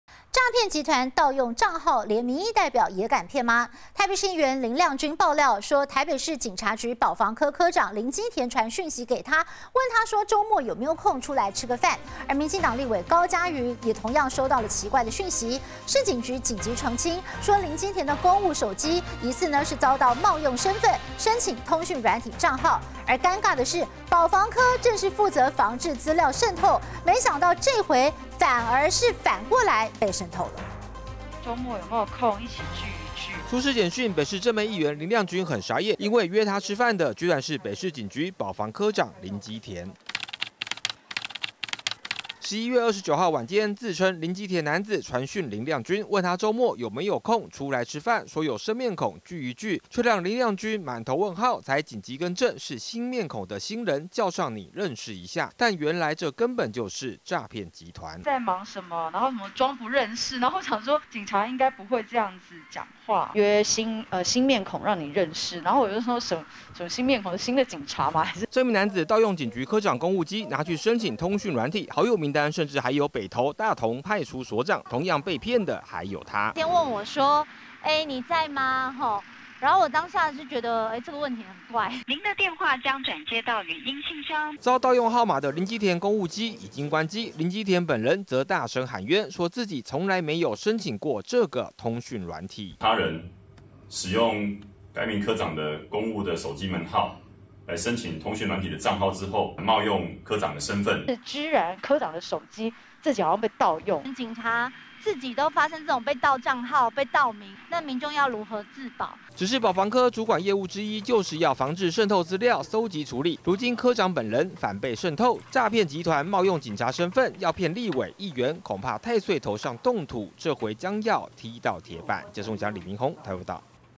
警局科長竟揪女議員吃飯!? 傻眼....帳號被盜了
詐騙集團盜帳號，連民意代表也敢騙嗎? 北市議員林亮君爆料，說北市警察局保防科長林基田傳訊息給她，問她「週末有沒有空、出來吃個飯」；民進黨立委高嘉瑜也同樣收到奇怪訊息，市警局緊急澄清，林基田的公務手機疑似遭詐騙集團冒用身份，申請通訊軟體帳號，尷尬的是，保防科正是負責防制資料滲透，這回卻反被滲透，也讓兩位民代同聲繾責。